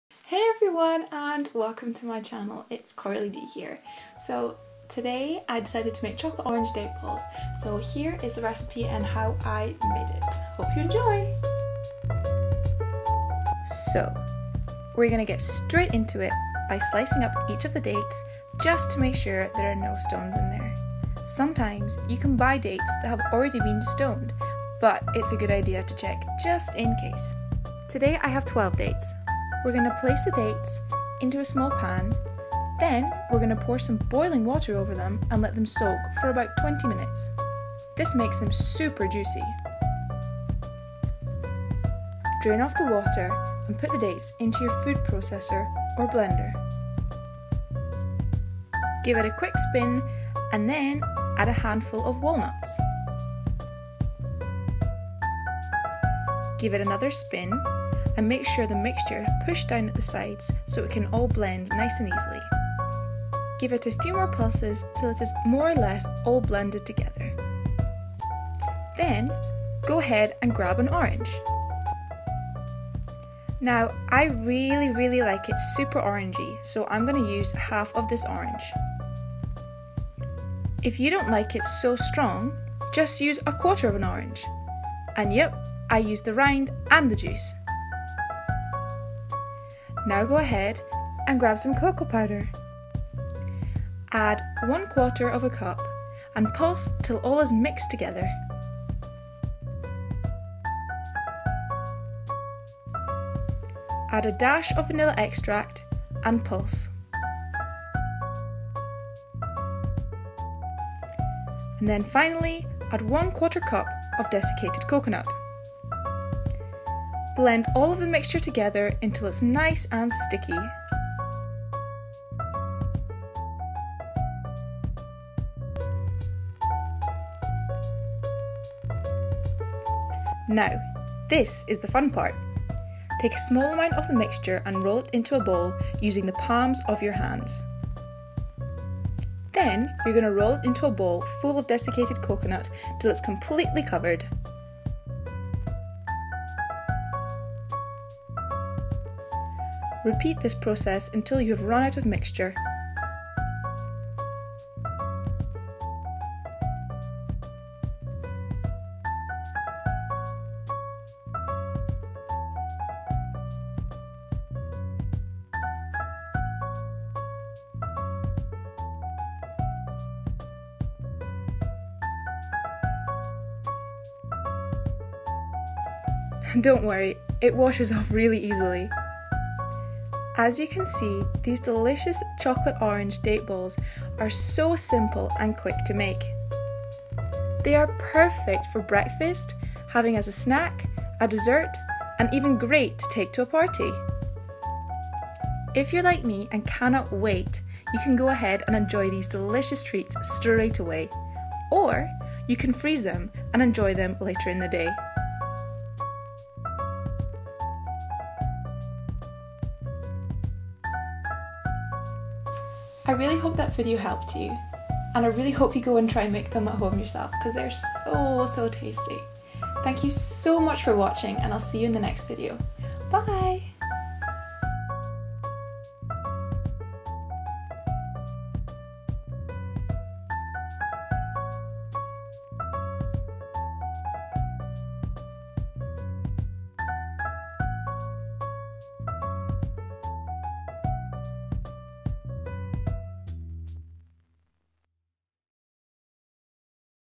Raw Vegan Chocolate Orange Balls Recipe
Today I made some Raw Vegan Chocolate Orange Date Balls and they were soooooooo damn tasty! I actually just went to the shops to buy more dates so I can make more! Please try these at home and tell me what you thought of them? These went down a treat and I cannot wait till I have more! Corals x